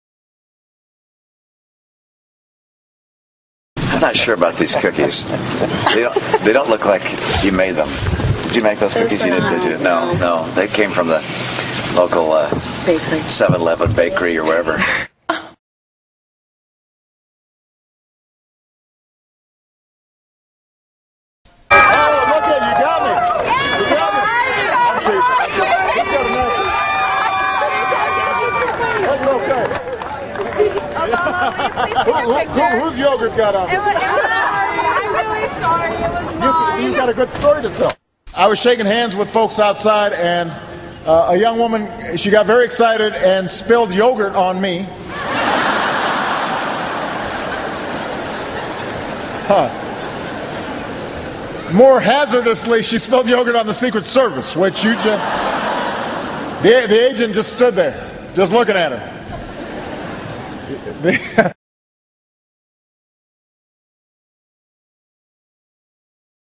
mitt romney Tells Woman "Your Cookies Suck"
Spoiled brat and unelectable presidential aspirant  mitt romney shows just how pompous, tactless, classless and out-of-touch he is when he insults gift-bearing supporters by telling them their cookies suck. And, conversely, President Barack Obama shows how in-tune with the People he is when a student accidentally spilled yogurt on him. President Barack Obama is comfortable around regular people, where classless reject romney is uncomfortable around regular people, and offers only insults in place of a simple gesture of thanks.